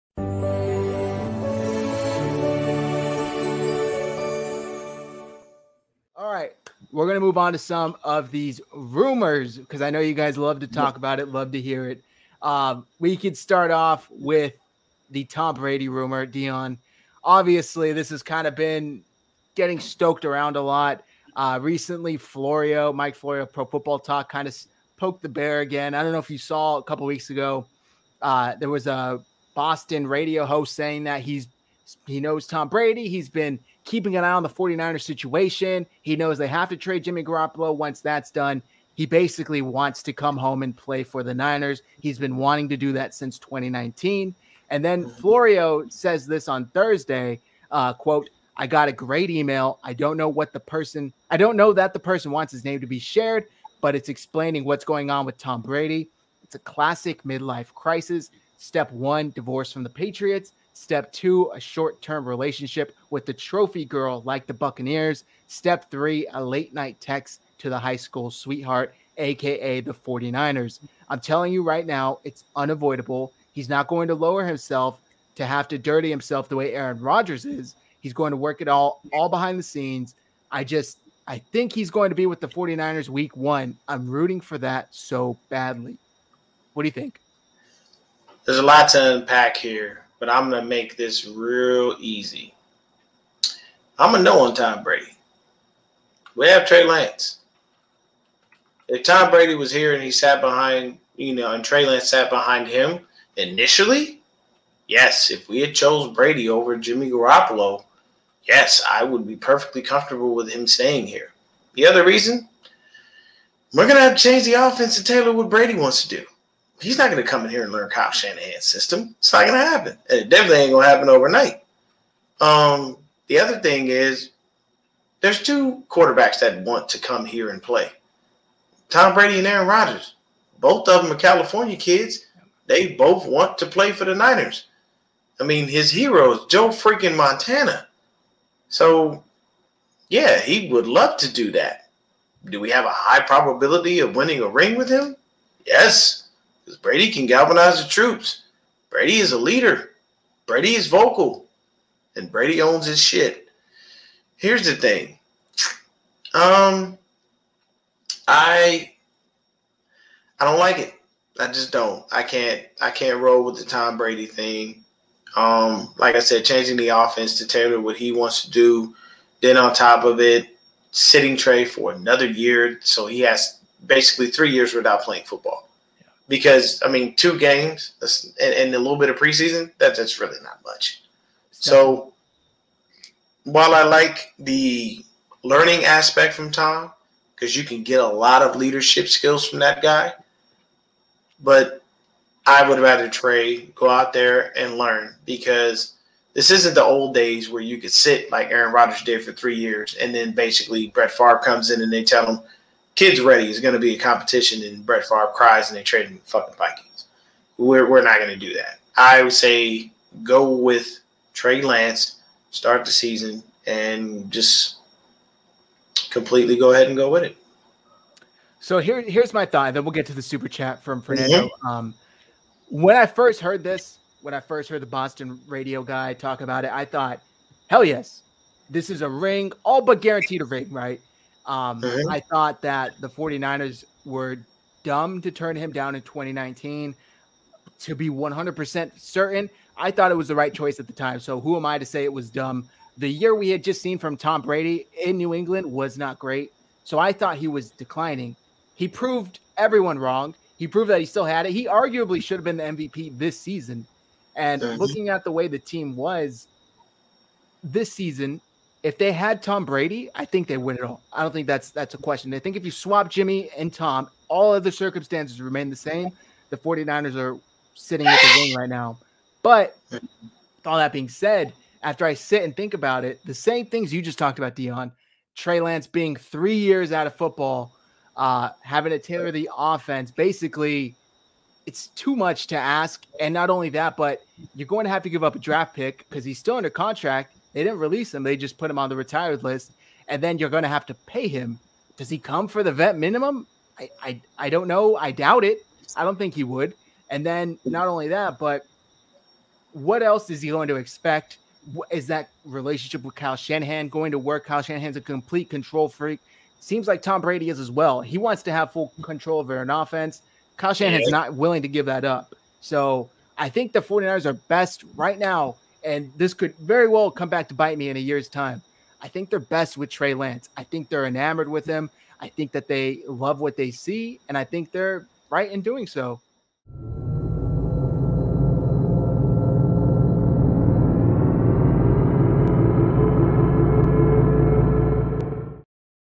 Tom Brady to the 49ers?
In today's livestream snippet of The Red & Gold Standard Podcast, Deon Johnson & Zach Hernandez discuss the rumors surrounding Tom Brady becoming a 49er!

Follow Deon Johnson & Zach Hernandez, RGS & 49ersHive on Twitter!